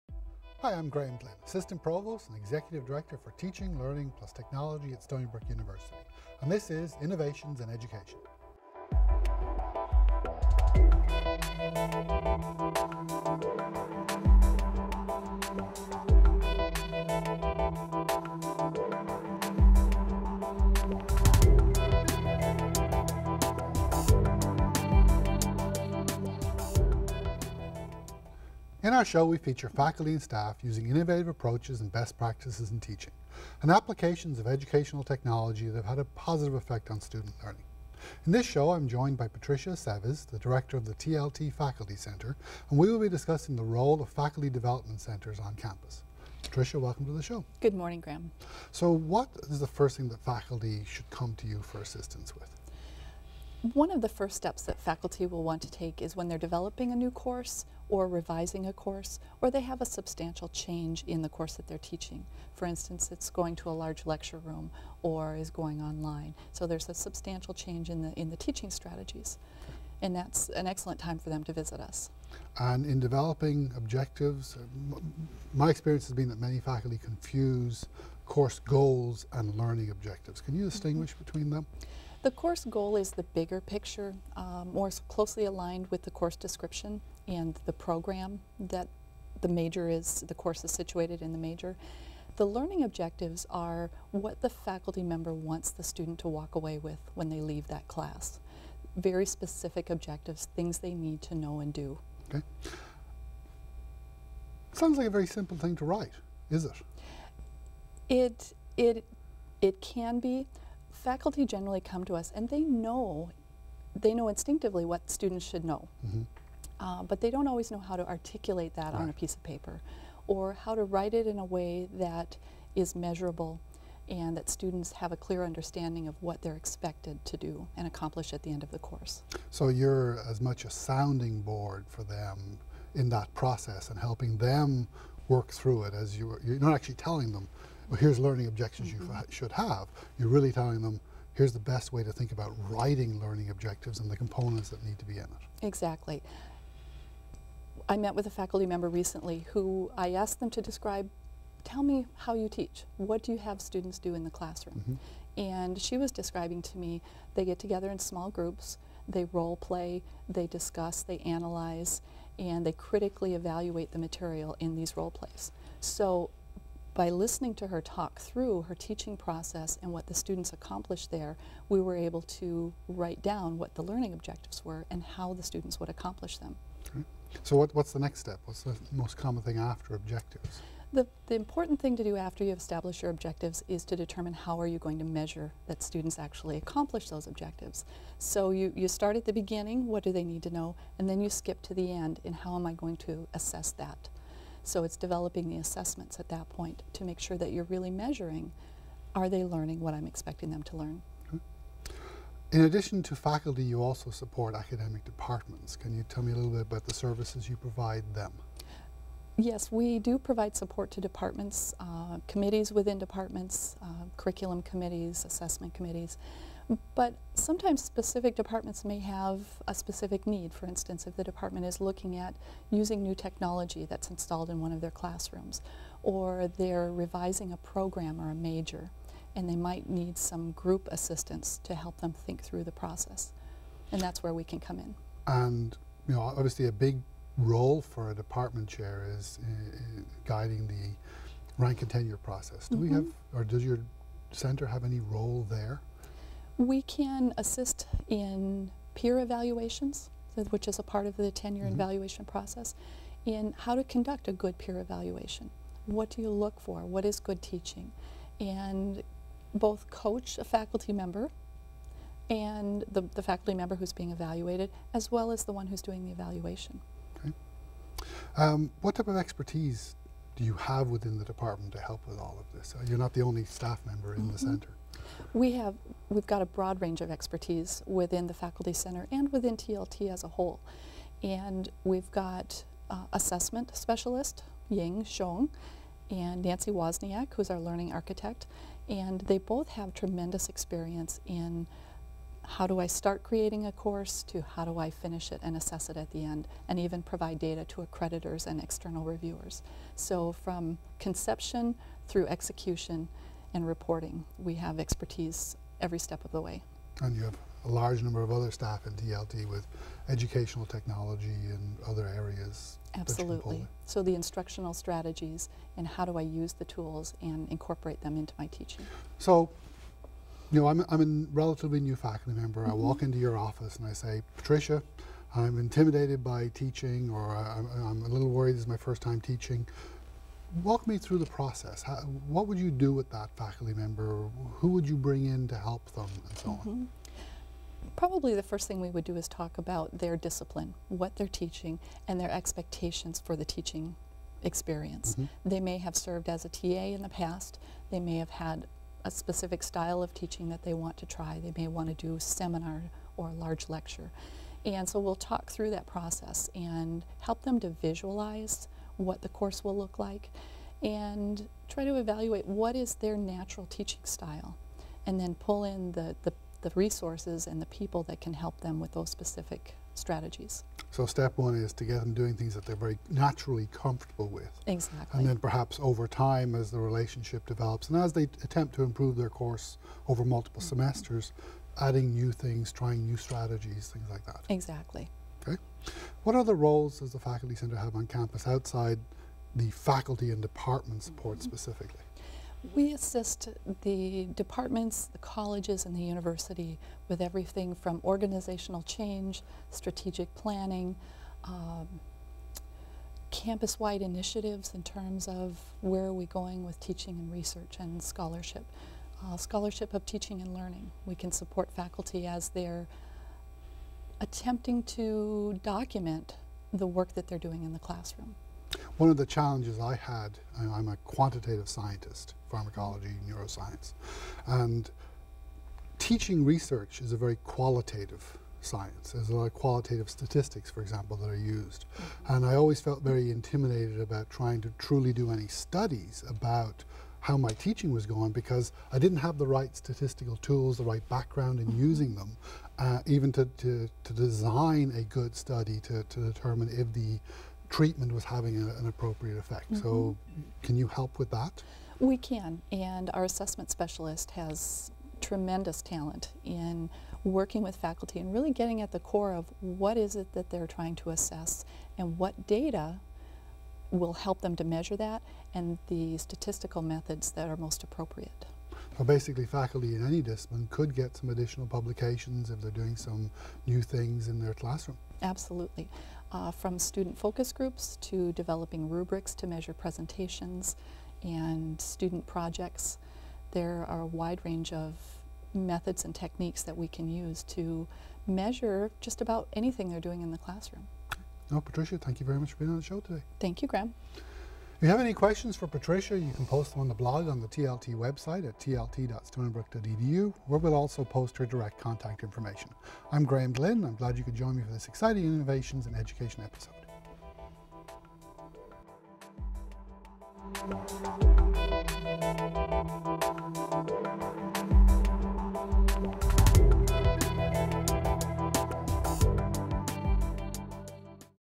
Aceves
Patricia Aceves, the director of The Faculty Center at Stony Brook, discusses backward course design and the benefits of clearly defining course learning goals and outcomes/objectives.  She also addresses how a teaching and learning center can assist faculty with developing pedagogical skills, and scholarship of teaching and learning (SOTL) skills.